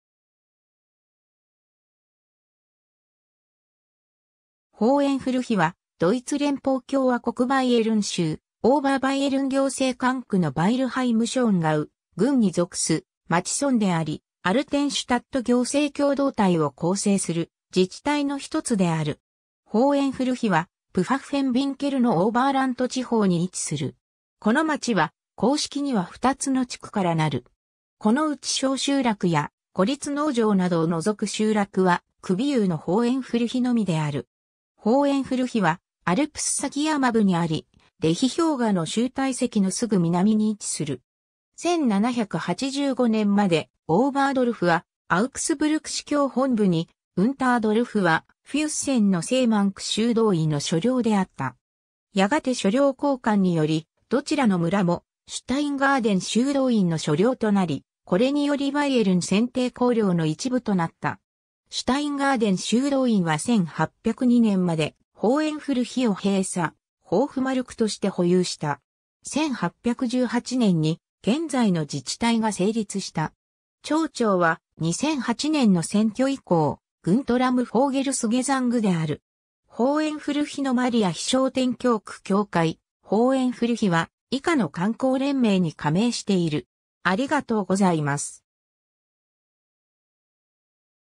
ホーエンフルヒ
ヴァイルハイム＝ショーンガウ郡
ホーエンフルヒ (ドイツ語: Hohenfurch) は、ドイツ連邦共和国バイエルン州オーバーバイエルン行政管区のヴァイルハイム＝ショーンガウ郡に属す町村（以下、本項では便宜上「町」と記述する）であり、アルテンシュタット行政共同体を構成する自治体の一つである。
 ホーエンフルヒはプファッフェンヴィンケルのオーバーラント地方に位置する。
 この町は、公式には 2つの地区 (Ort) からなる。
このうち小集落や孤立農場などを除く集落は、首邑のホーエンフルヒのみである。
 ホーエンフルヒはアルプス前山部にあり、レヒ氷河の終堆石のすぐ南に位置する。
 1785年までオーバードルフ（上の村）はアウクスブルク司教本部に、ウンタードルフ（下の村）はフュッセンの聖マンク修道院の所領であった。
やがて所領交換により、どちらの村もシュタインガーデン修道院の所領となり、これによりバイエルン選帝侯領の一部となった。
シュタインガーデン修道院は1802年までホーエンフルヒを閉鎖ホーフマルクとして保有した。
1818年に現在の自治体が成立した。
 町長は、2008年の選挙以降、グントラム・フォーゲルスゲザング (CSU) である。
 ホーエンフルヒのマリア被昇天教区教会 ホーエンフルヒは、以下の観光連盟に加盟している。